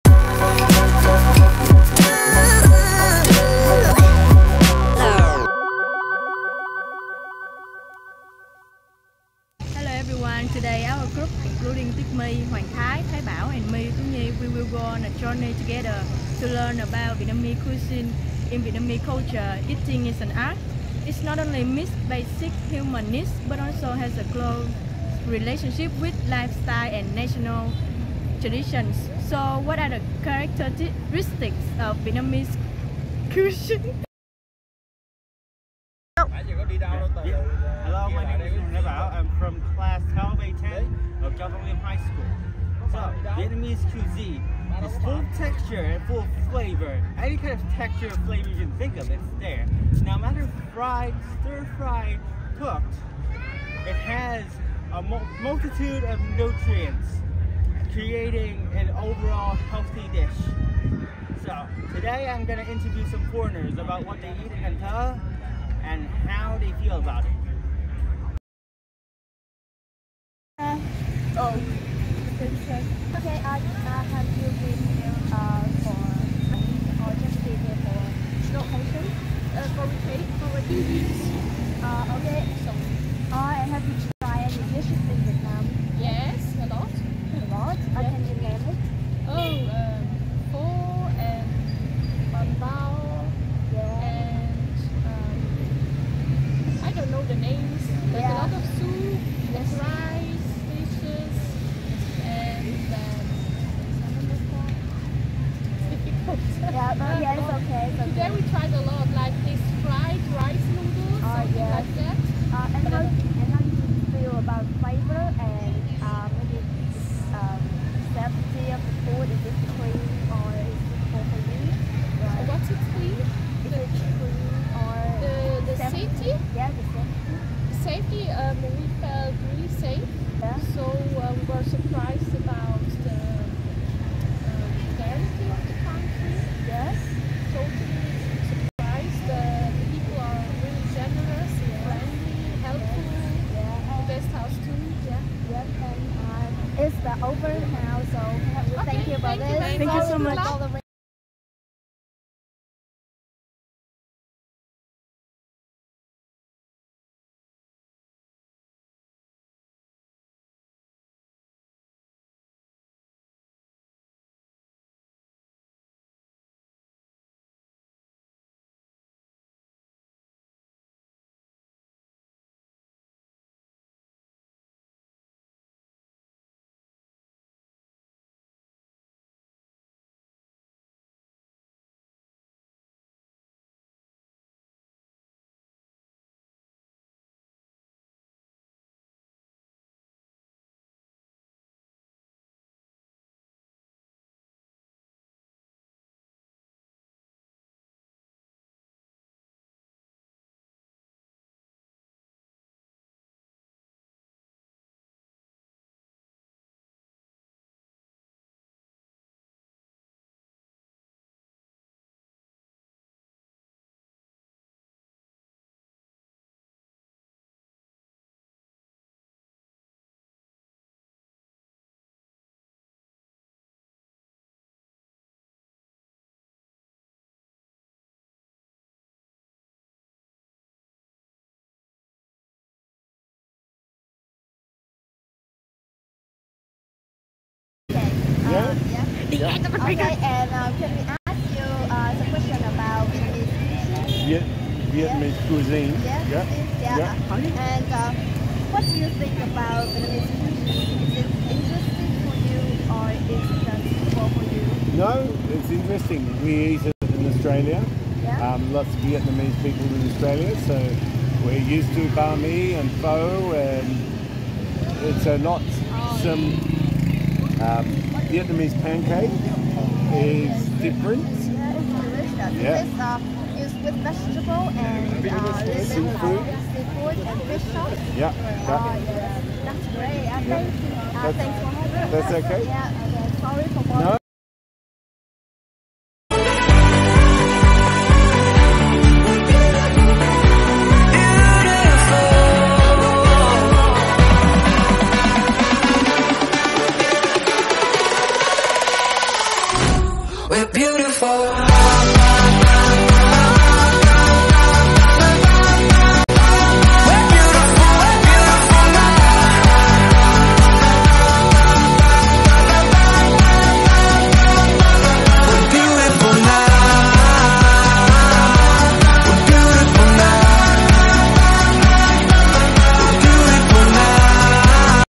12A10_Becoming a reporter
Xin chào.
Video Clip này là sản phẩm tham dự cuộc thi "Becoming a reporter", một trong những hoạt động của Tháng Ngoại Ngữ tại trường THPT Châu Văn Liêm năm học 2023_2024.
Cảm ơn đã xem clip. Vui lòng Like và Share video này nếu thấy hữu ích.